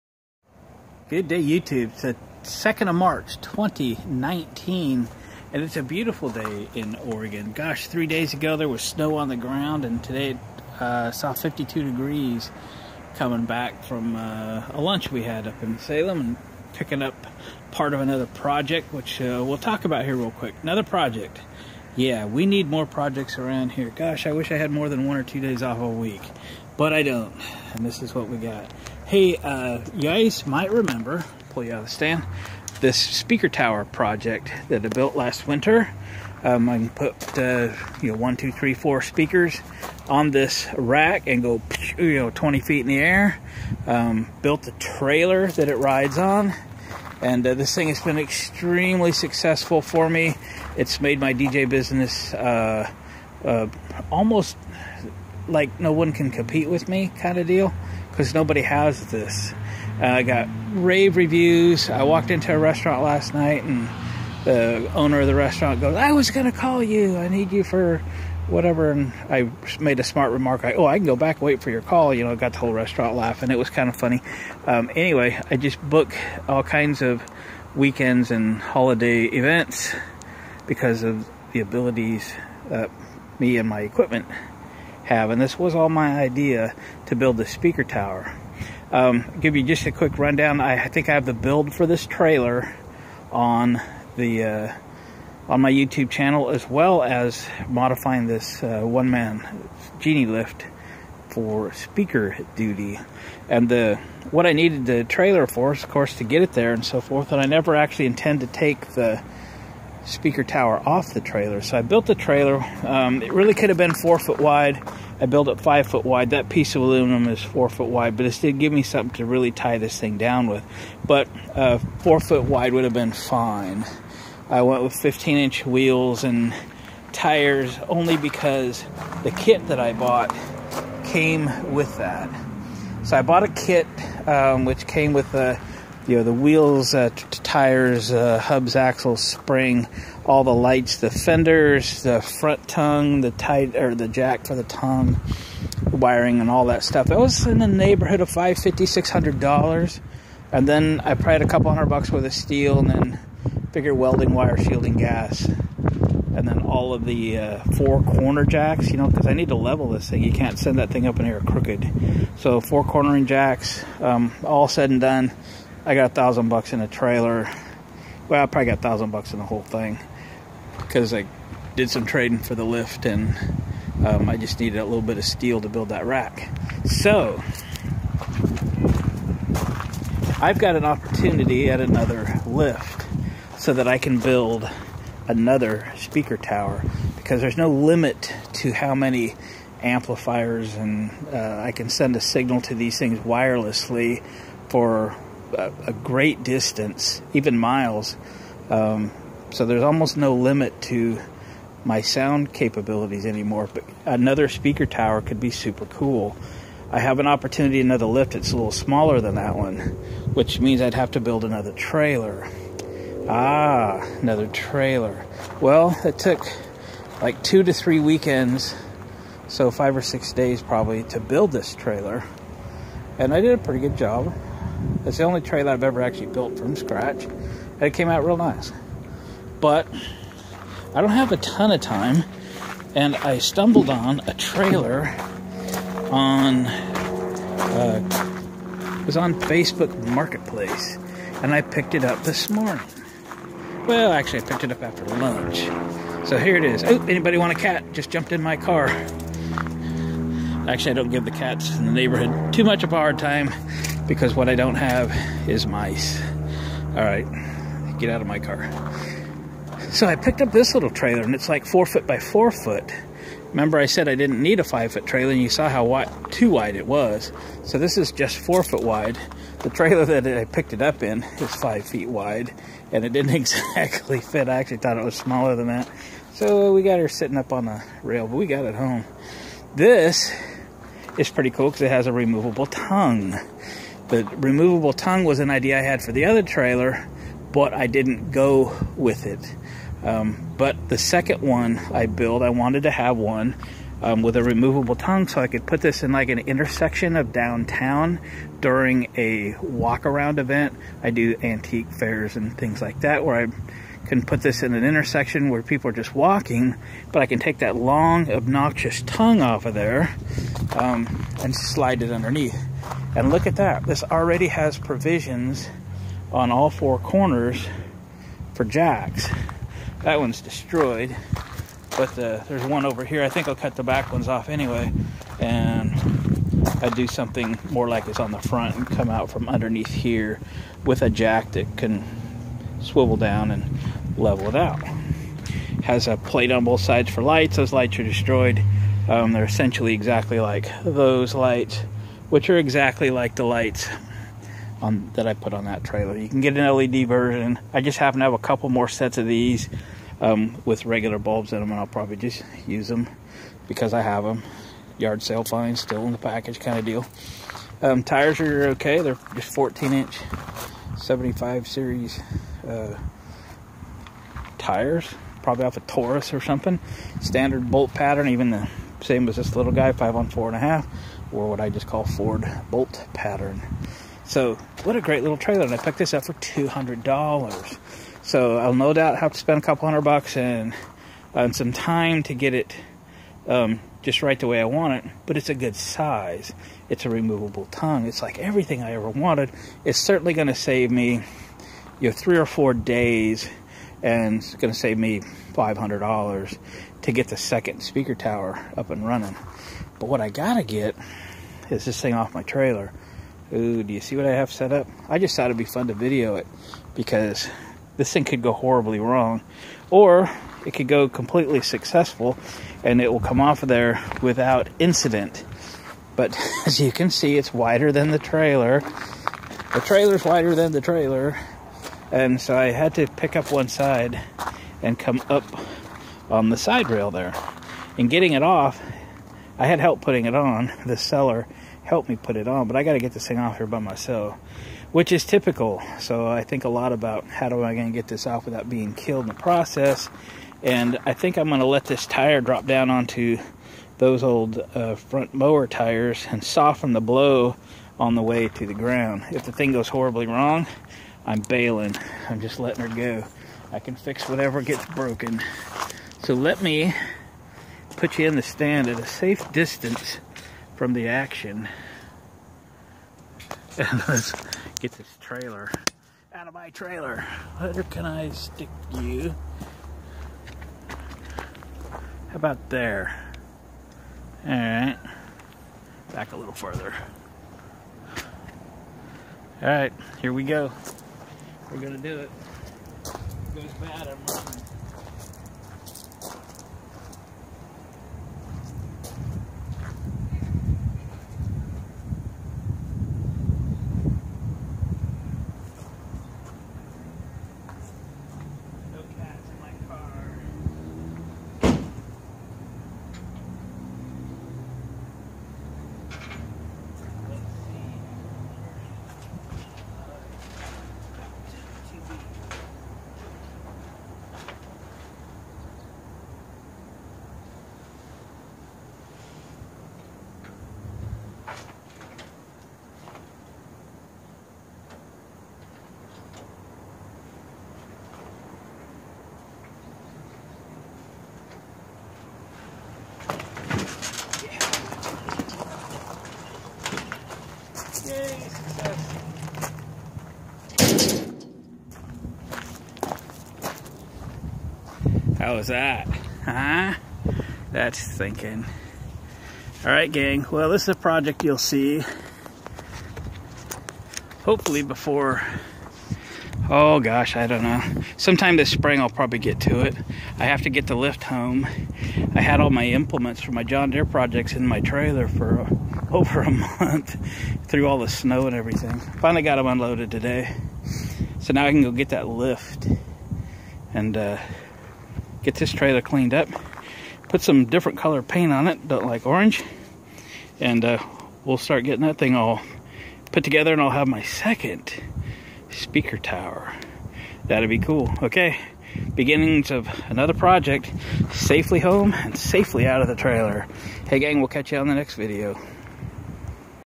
Bringing home parts for an upcoming project the best way I know how without cops giving me a ticket!
Now I have to unload my new old trailer off my trailer without getting killed in the process...